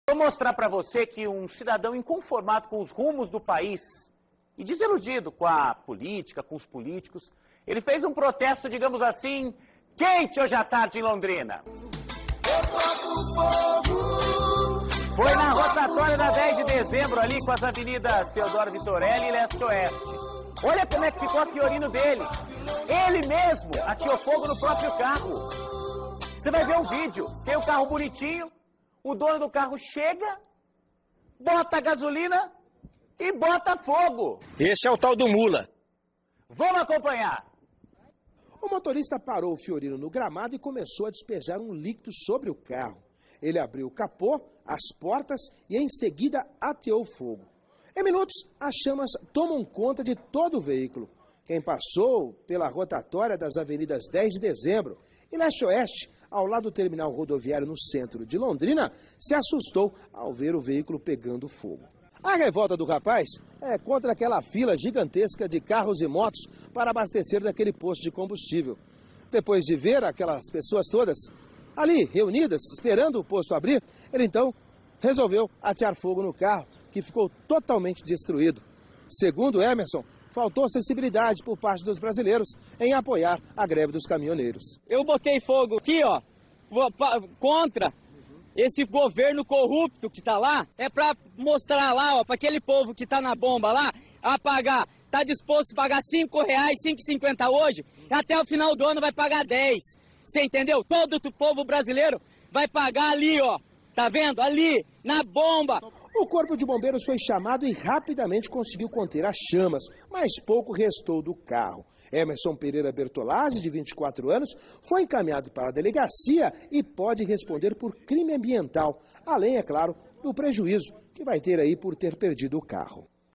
Homem se revolta com fila e ateia fogo em próprio veículo
Desiludido com a política e com o país, um civil colocou fogo no próprio carro na rotatória da 10 de Dezembro com a Avenida Theodoro Victoreli.